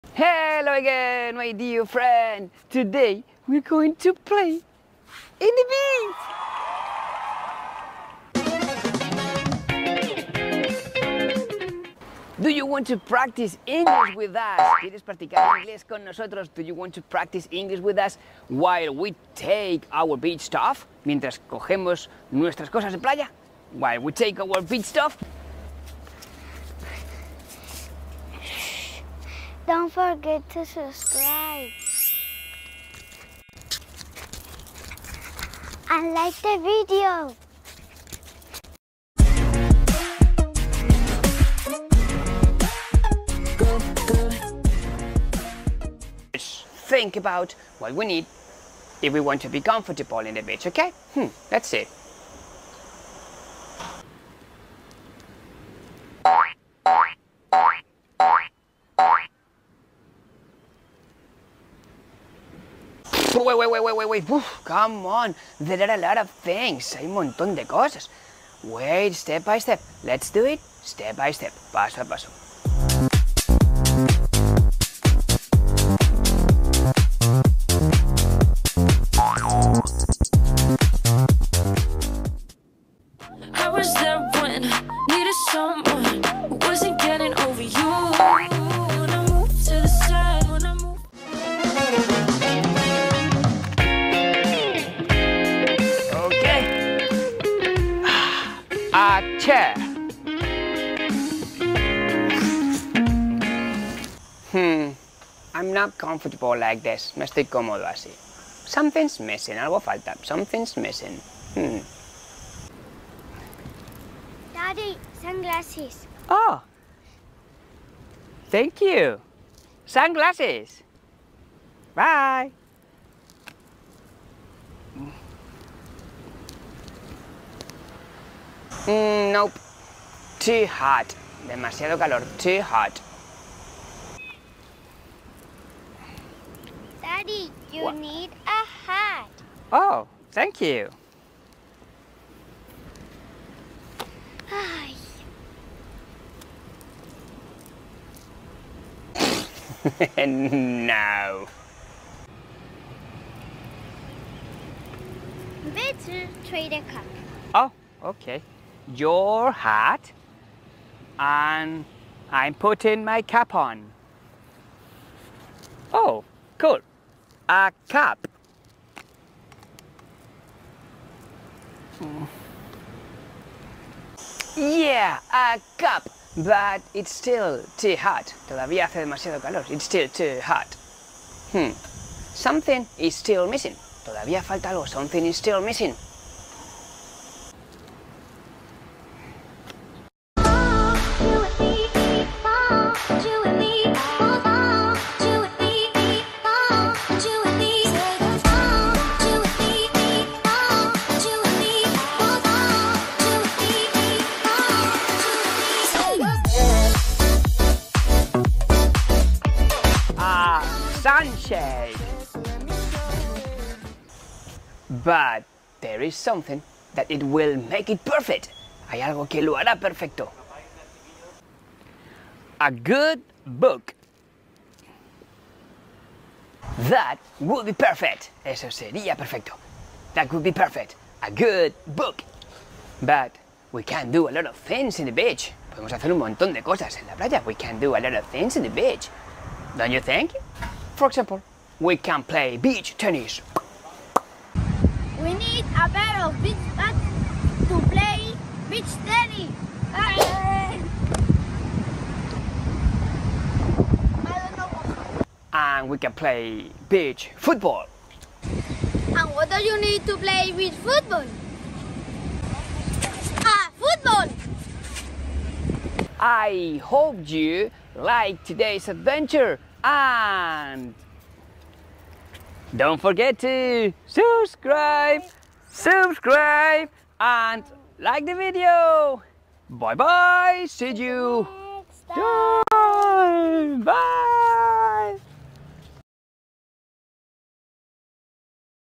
🏝️Beach Vocabulary🏖️. Vocabulario para la playa. Inglés para niños
Aprende con nosotros todo ese vocabulario esencial para el verano y para ir a la playita!!!